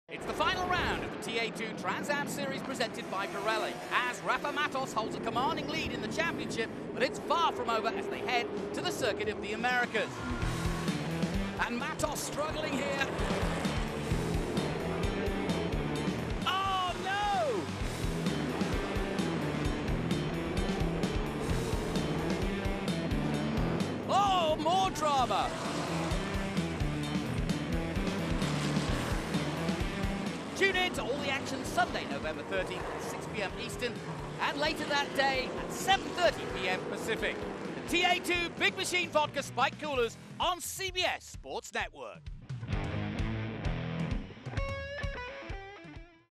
Watch the TA2 Race from COTA tomorrow at 6pm on CBS Sports Network!
The GoTransAm races at Circuit of The Americas were off the charts -- and they both air tomorrow on CBS Sports Network! Watch the TA2 Race presented by Big Machine Vodka Spiked Coolers when it airs at 6pm ET (or tune in at 2pm ET to watch the TA/XGT/SGT/GT Race, too!). It all happens tomorrow, Sunday, November 13!